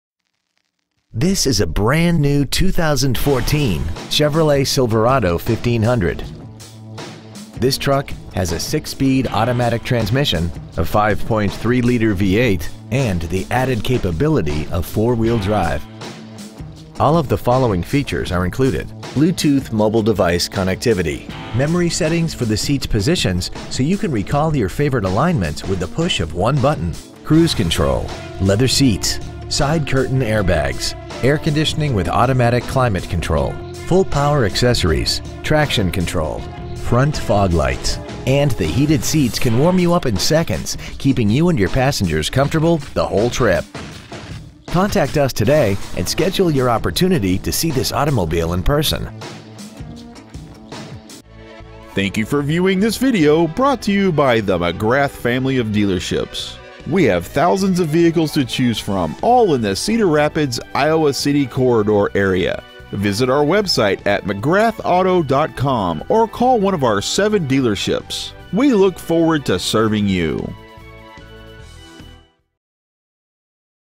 2014 Chevrolet Silverado 1500 Cedar Rapids | Iowa City IA
We have been honored to serve the Cedar Rapids/Iowa City Area IA area , we promise that your experience at our dealership will exceed your expectations ! Year : 2014 Make : Chevrolet Model : Silverado 1500 Engine : 5.3L V8 Trans . : Automatic 6-Speed Exterior : Summit White Interior : Jet Black/Dark Ash w/Leather Appointed Seat Trim Stock : C141282 McGrath Auto Group Cedar Rapids/Iowa City Area , IA